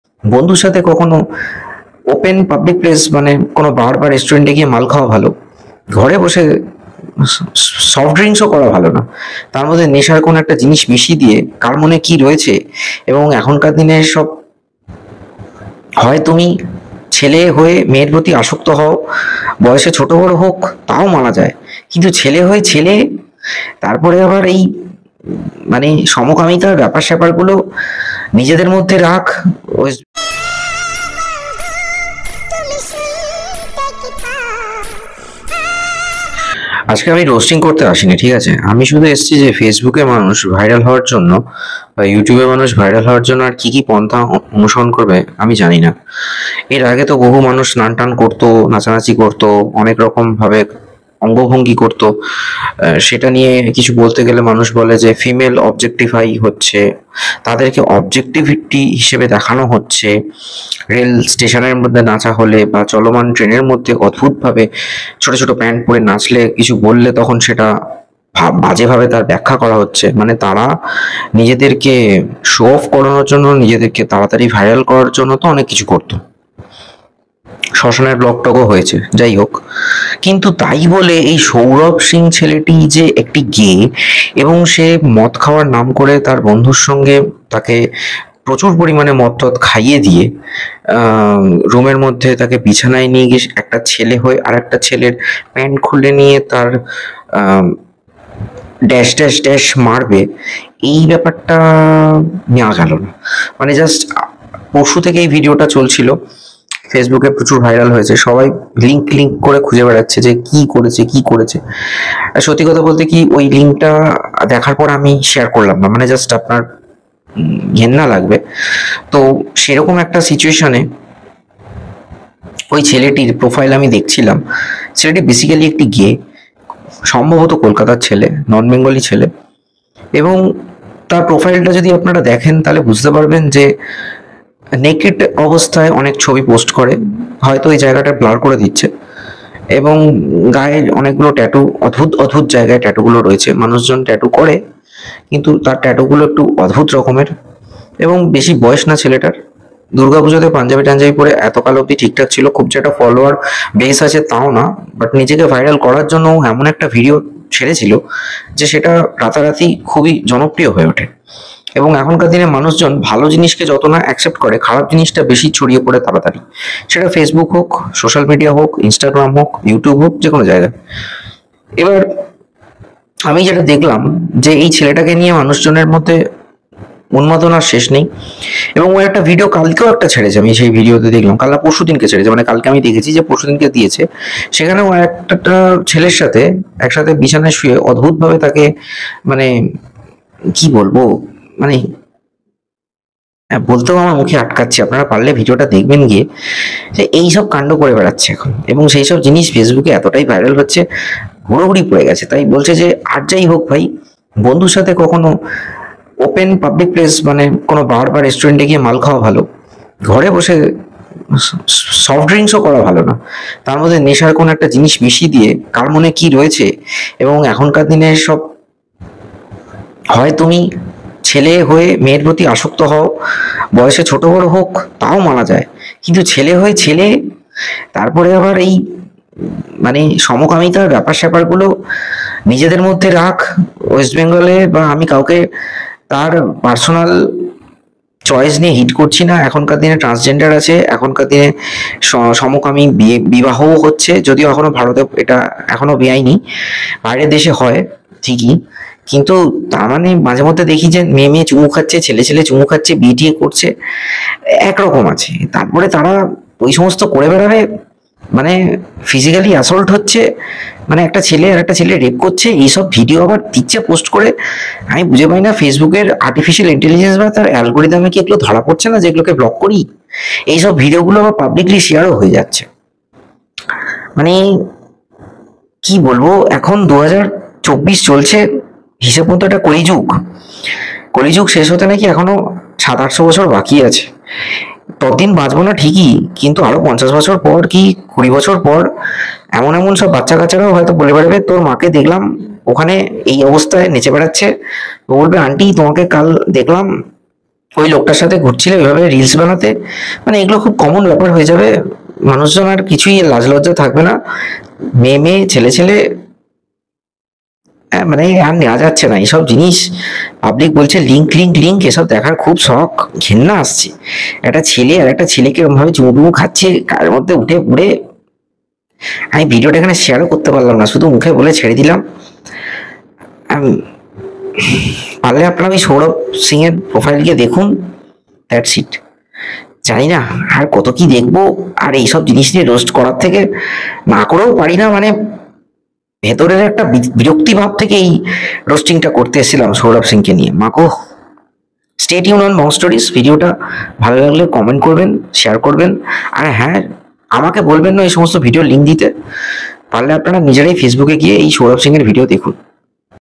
SOURAV SINGH Gay VIRAL POST|ছেলে বন্ধু হয়ে আরেক বন্ধুর পেছন মারলো|Gey sexual porn vedio shoot viral
ছেলে বন্ধু হয়ে আরেক বন্ধুর পেছন মারলো|ছেলে বন্ধু হয়ে আরেক বন্ধুর সাথে যৌন সম্পর্ক
 সম্প্রতি সমকামিতা হিসেবে পরিচিত সৌরভ বলে একটি ছেলে মদ খাইয়ে তার বন্ধুর সাথে যৌন সম্পর্ক স্থাপন করেছে